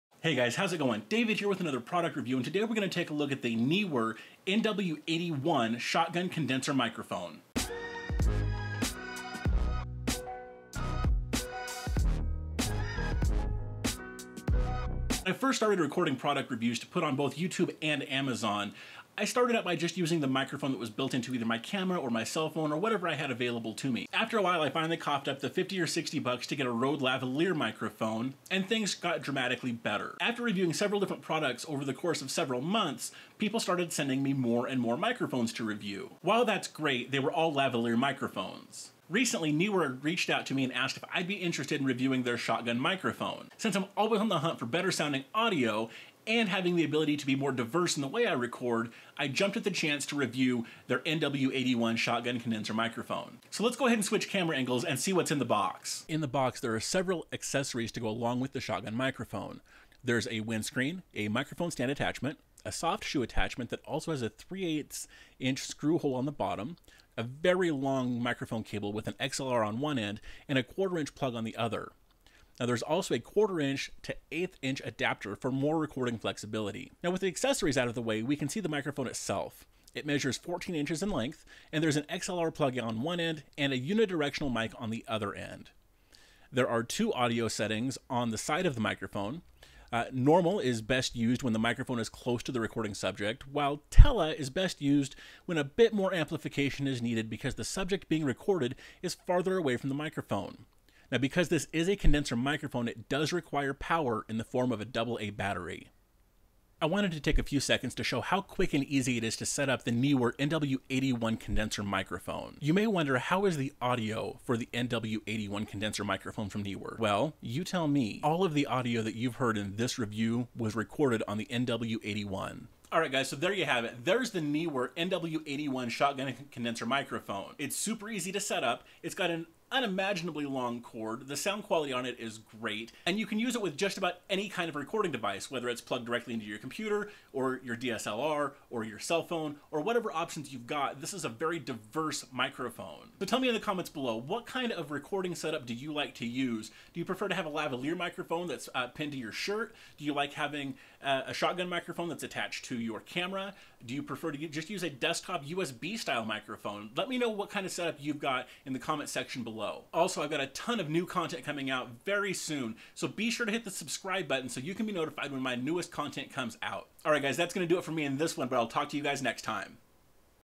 Neewer NW-81 Shotgun Condenser Microphone
This is a perfect compliment to my video recording. It's super simple to setup and sounds great.

In the package you get:

1 x Shotgun Condenser Microphone
1 x Wind Sock
1 x Microphone Stand Adapter
1 x Softshoe Adapter
1 x 20' Microphone cable with XLR and 1/4" plug
1 x 1/4" to 1/8" adapter

The microphone requires a AA battery for power and the battery isn't included.

There are 2 settings on the microphone: Normal and Tele. Normal is for situations where the person being recorded is close to the microphone whereas the Tele setting adds a bit more amplification to the mic to pick up the subject a little better.

If you're looking to step up your audio recording game, definitely take a look at the Neewer NW-81!